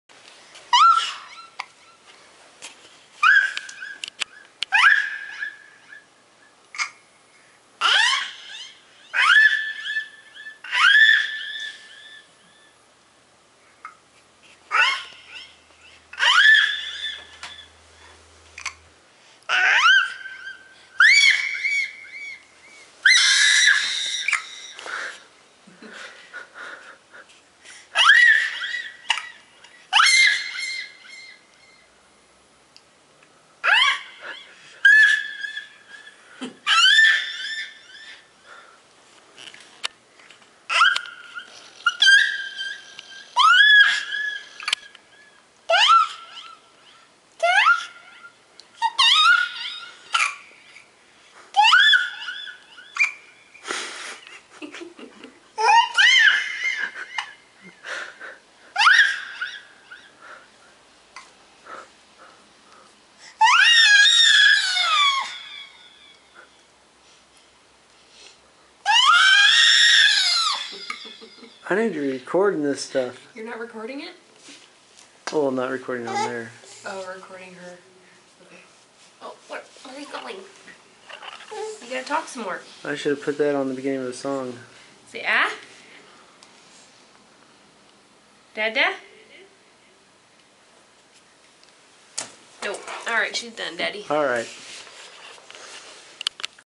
Baby Anabelle discovers delay
My 11 month old discovers delay for the first time! :)